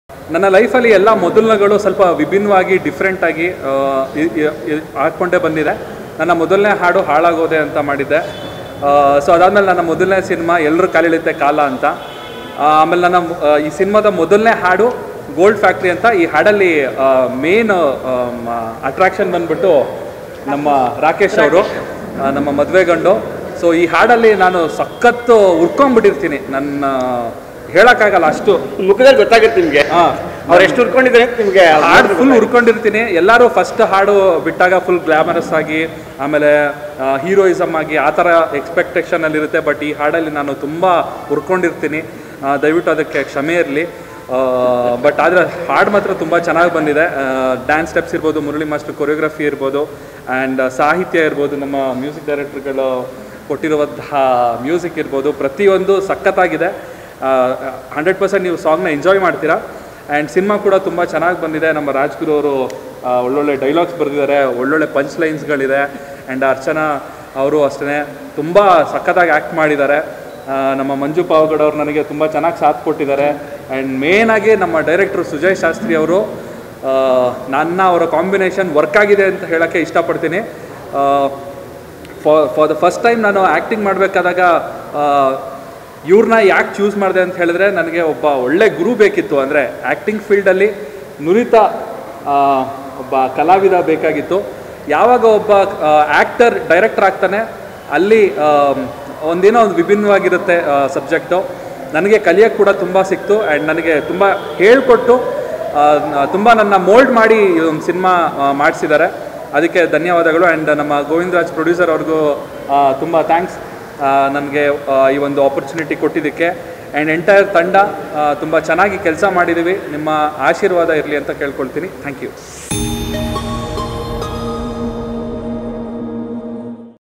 'ಅವಕಾಶ ಕೊಟ್ಟ ನಿರ್ಮಾಪಕರಿಗೆ ಥ್ಯಾಂಕ್ಸ್‌!' - ಚಂದನ್‌ ಶೆಟ್ಟಿ | 'Elra Kaaleliyatte Kaala' Film
'ಅವಕಾಶ ಕೊಟ್ಟ ನಿರ್ಮಾಪಕರಿಗೆ ಥ್ಯಾಂಕ್ಸ್‌!' - ಚಂದನ್‌ ಶೆಟ್ಟಿ | 'Elra Kaaleliyatte Kaala' Film | Kannadamojo360